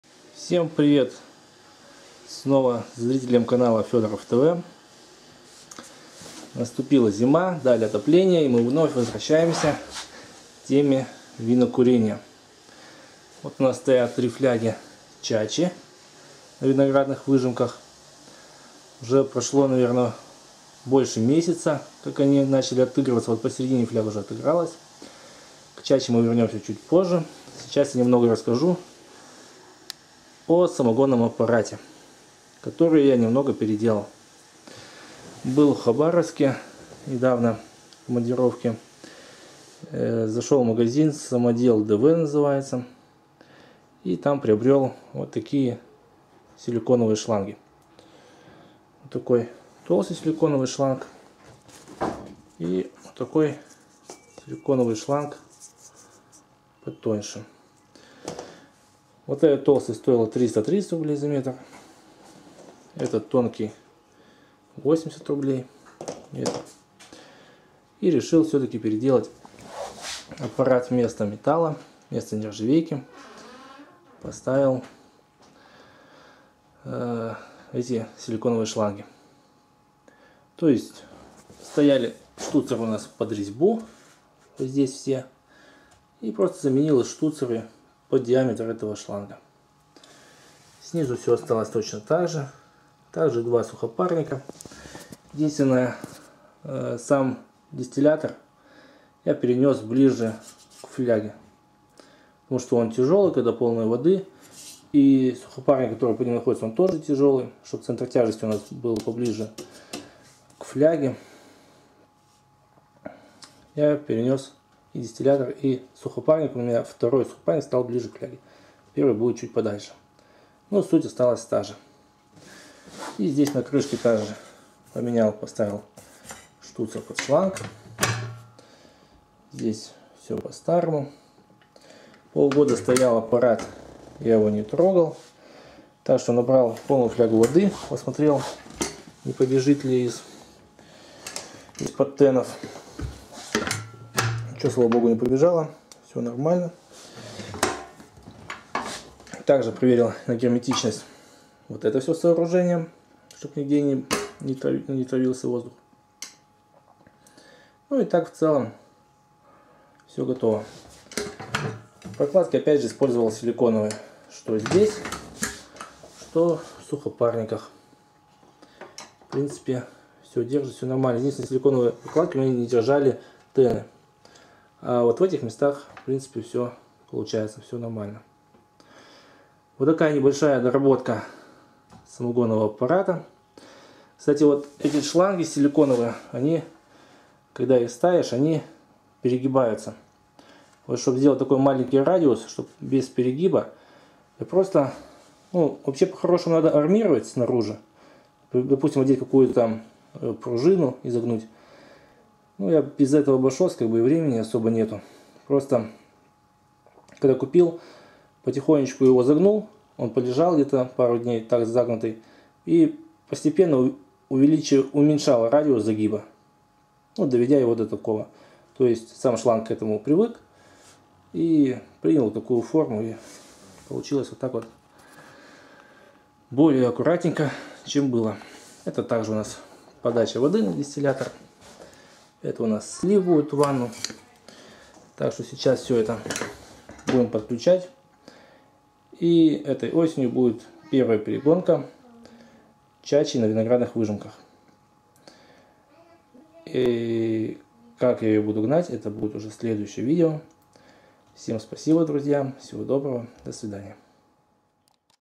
Самогонный аппарат с силиконовыми шлангами теперь!
Поставил на свой самогонный аппарат силиконовые шланги, не сказать что б прям это была необходимость, но поставил... Вот что получилось!

Если вы посмотрели видео и у вас появились вопросы, пожелания или предложения то:

Особенно благодарен за критику, ведь она делает канал лучше! )

Или просто на карту: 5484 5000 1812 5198